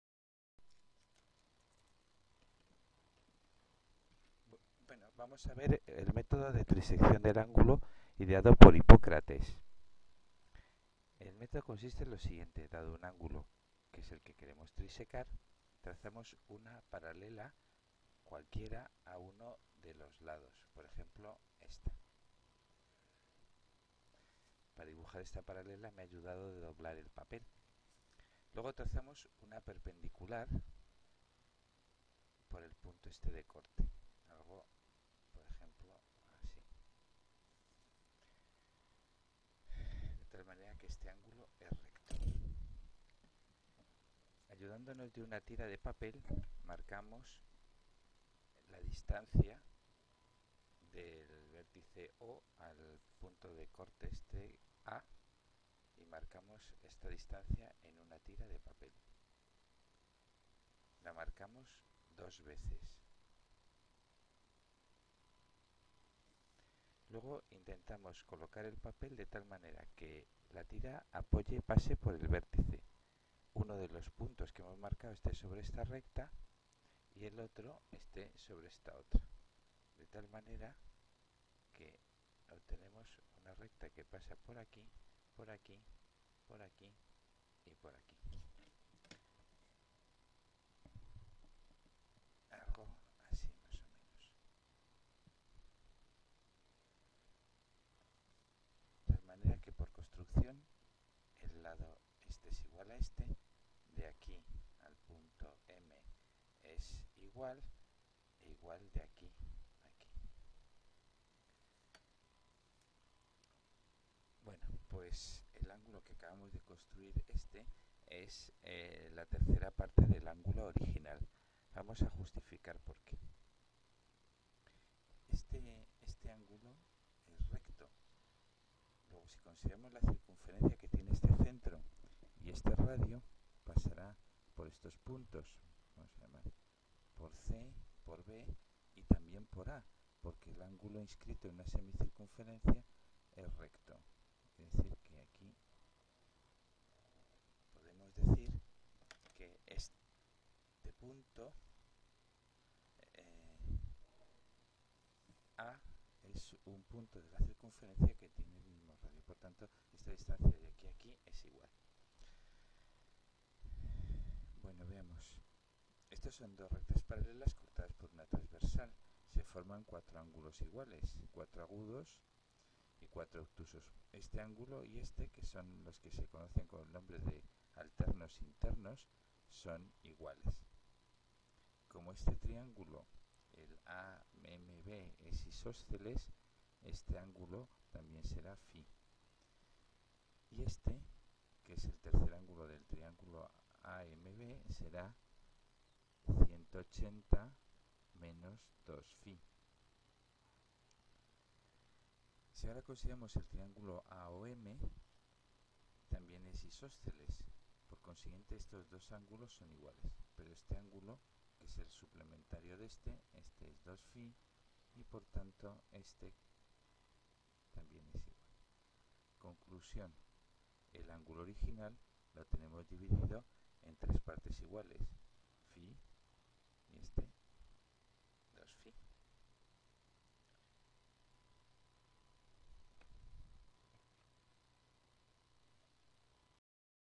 triseccion de hipocrates
Trisección de un ángulo utilizando el método de Hpócrates